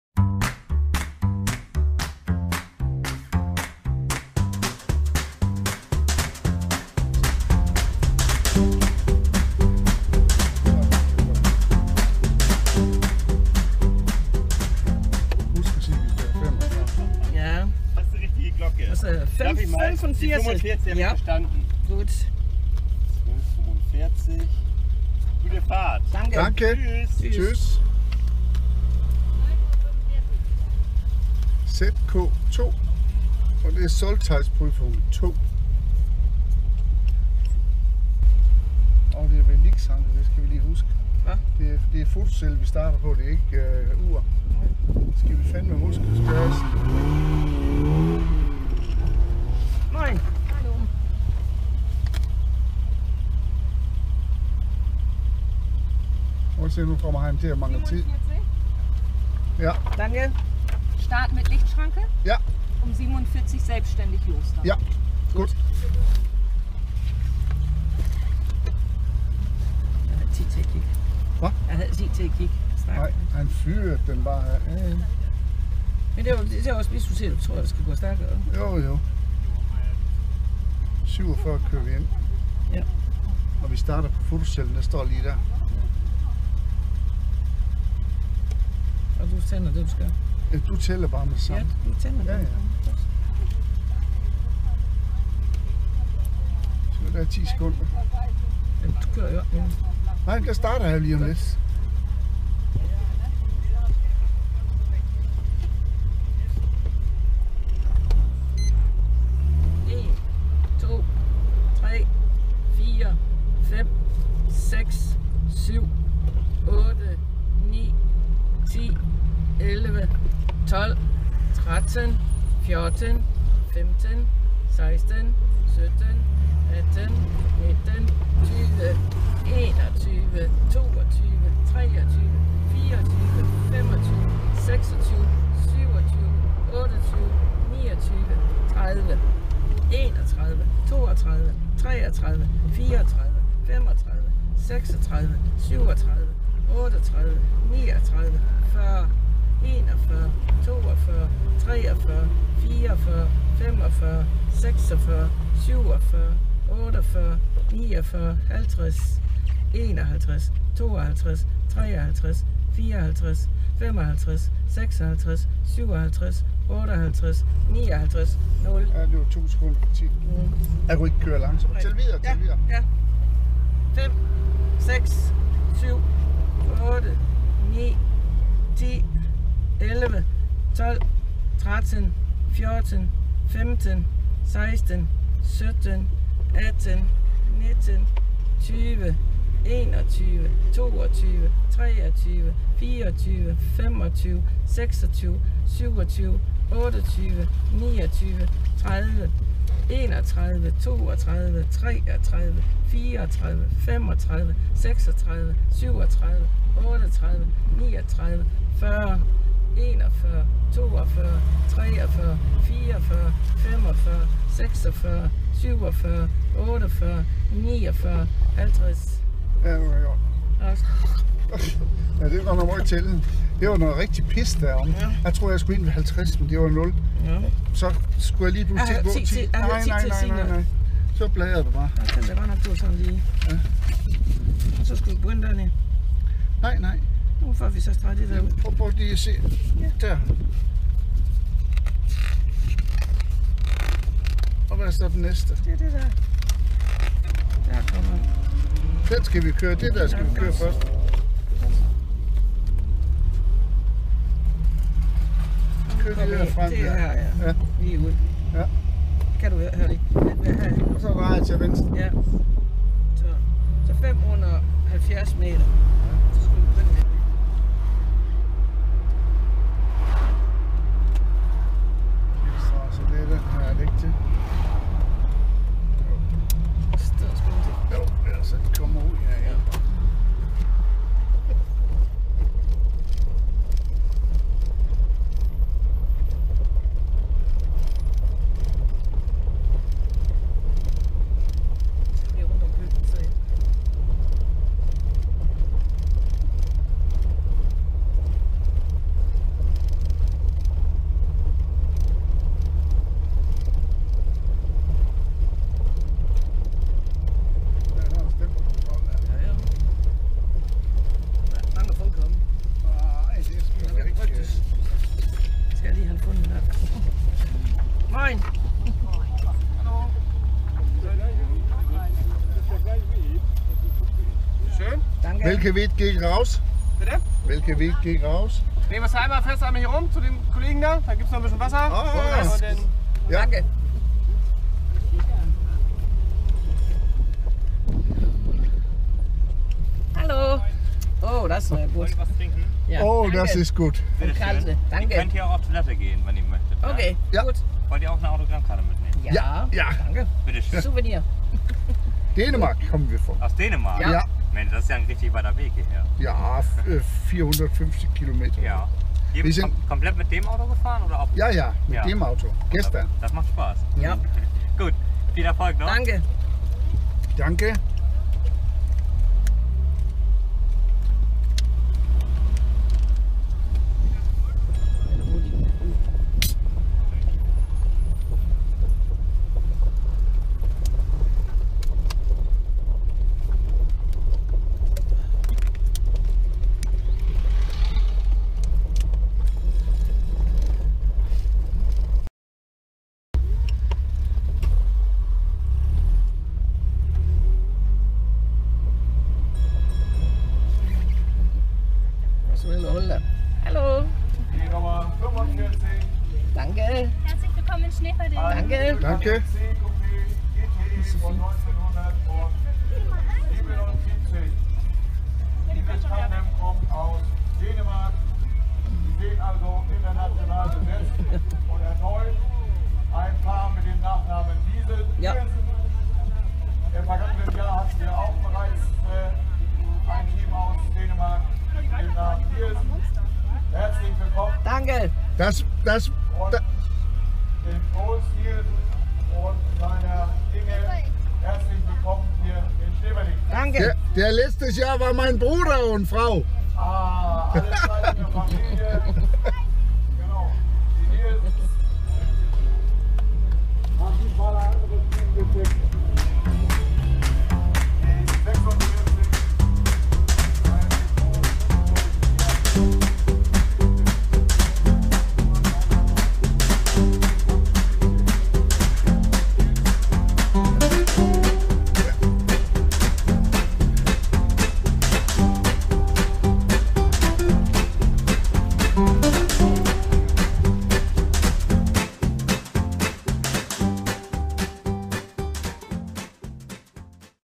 Heide Classic Video1 2019
WertungsPrüfung 2 - Heide Classic, 2019 TeamOldTime / Troels og Inge / Opel Kadett Cpupe C GT/E